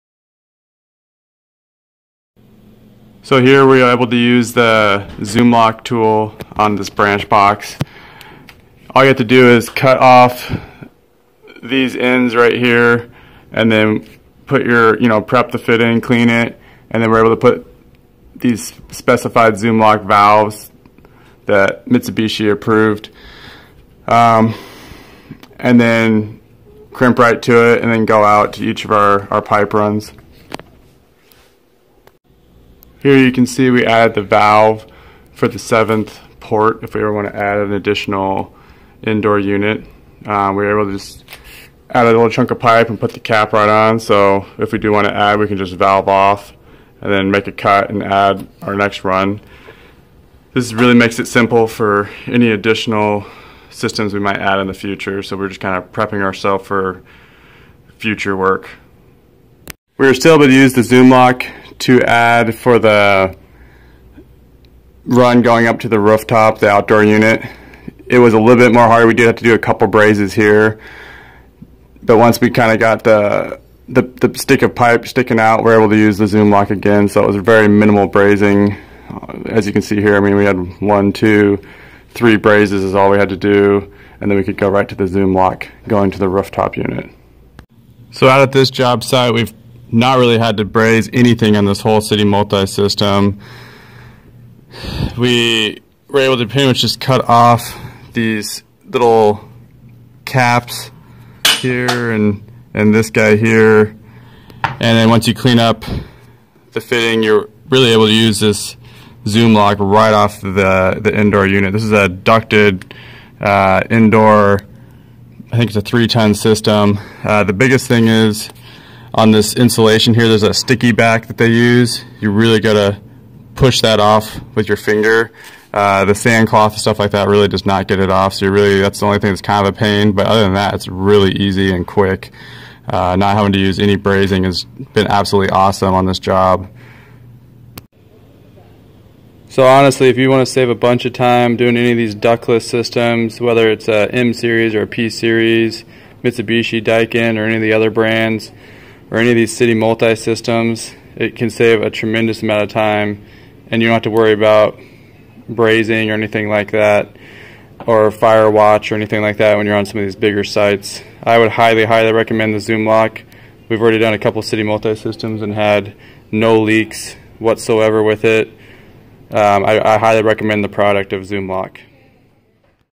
Zoomlock: Tips and Tricks for faster Mitsubishi City Multi install
We used the zoomlock flame free tool on our city multi install. This video will show you the results on how it went. It will also teach you some tips and tricks to make it go smoother and mistakes we learned along the way. It shows you the Zoomlock valves in action on the branch box. The zoomlock is a great tool and helps reduce fire watch on your work site. It also keep your from having to purge the system with nitrogen when you braze.